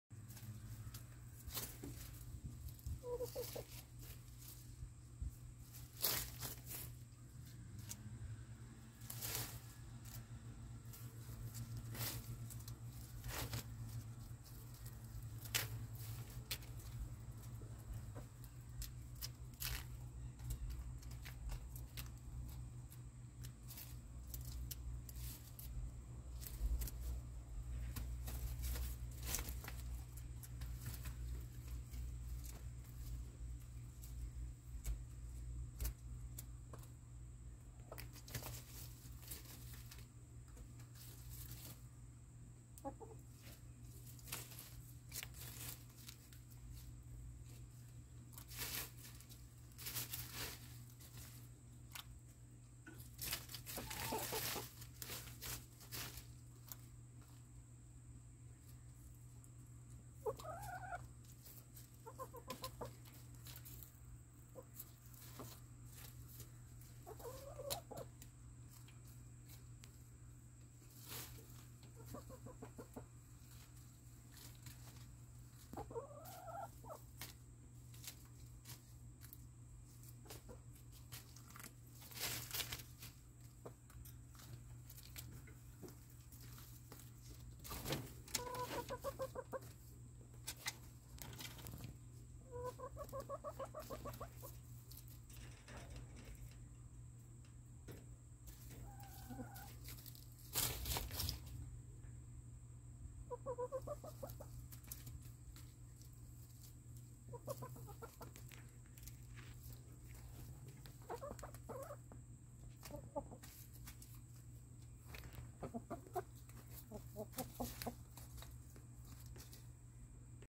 Feeding Chickens bead fern, grape leaves, tomato leaves...such a happy bunch!
Buff Orpington and ISA Brown chickens having lunch, feasting on bead fern, grape leaves, tomato leaves...such a happy bunch!